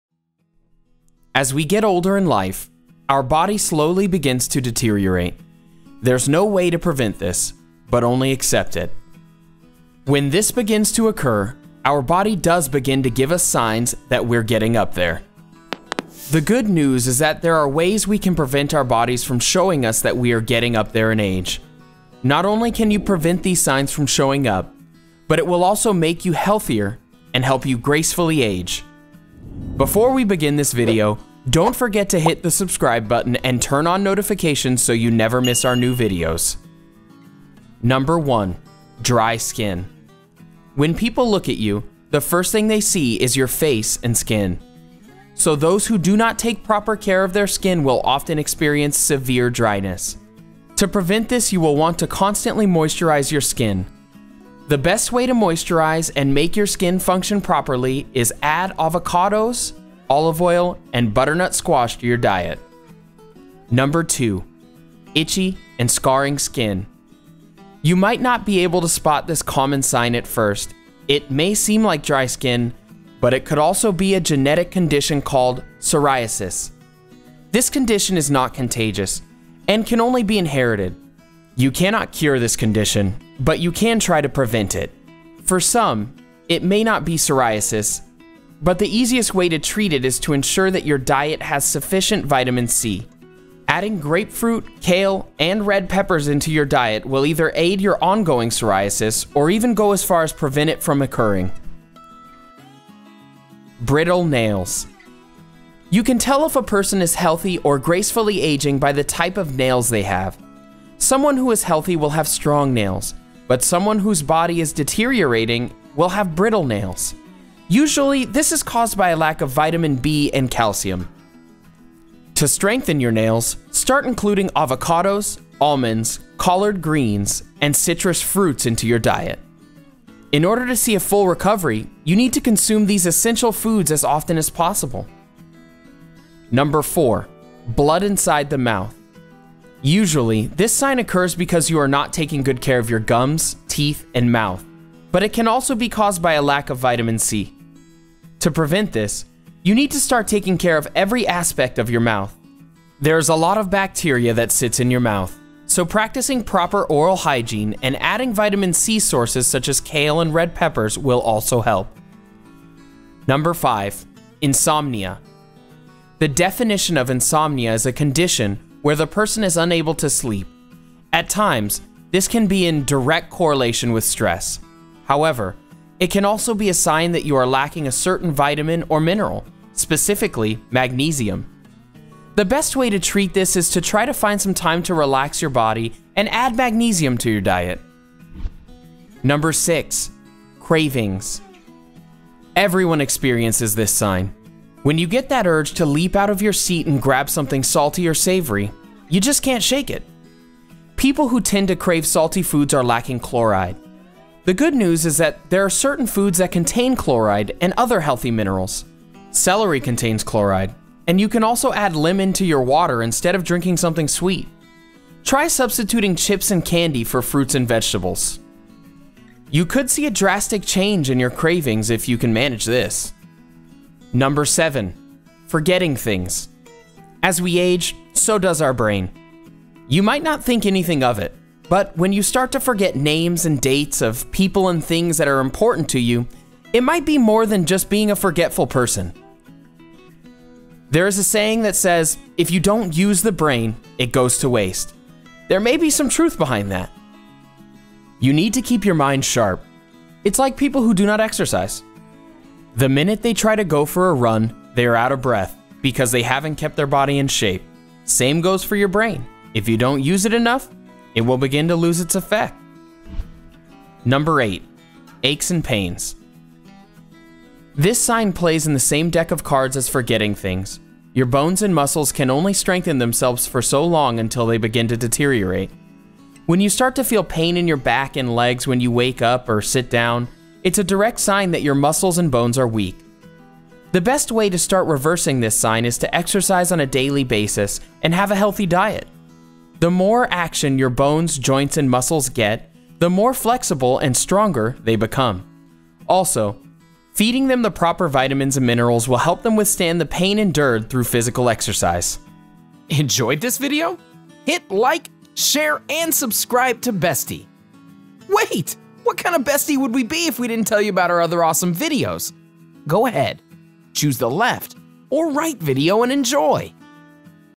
Here Are A Few Common Signs That Show Your Body Is Slowly Deteriorating
These signs show that your body is slowly deteriorating and aging. As we get older in life, our body slowly begins to deteriorate. There is no way to prevent this but only accept it. When this begins to occur, our body does begin to give us signs that we’re getting up there.

The good news is that there are ways we can prevent our bodies from showing us that we are getting up there in age. Not only can you prevent these signs from showing, but it will also make you healthier and help you gracefully age.

Timestamps:
Intro - 0:00
1 - Dry Skin: 0:55
2 - Itchy/Scarring Skin: 1:21
3 - Brittle Nails: 2:02
4 - Blood Inside the Mouth: 2:34
5 - Insomnia: 3:01
6 - Cravings: 3:27
7 - Forgetting Things: 4:03
8 - Aches and Pains: 4:42

Summary:
When people look at you, the first thing they see is your face/skin. So those who do not take proper care of their skin will often experience.

You might not be able to spot this common sign at first. It may seem like dry skin, but it could also be a genetic condition called Psoriasis. This condition is not contagious and can only be inherited.

You can tell if a person is healthy or gracefully aging by the type of nails they have. Someone who is healthy will have strong nails, but someone whose body is deteriorating will have brittle nails. Usually, this is caused by a lack of vitamin B and calcium.

Usually, this sign occurs because you are not taking good care of your gums, teeth, and mouth. But it can also be caused by a lack of vitamin C.

The definition of insomnia is a condition where the person is unable to sleep. At times this can be in direct correlation with stress.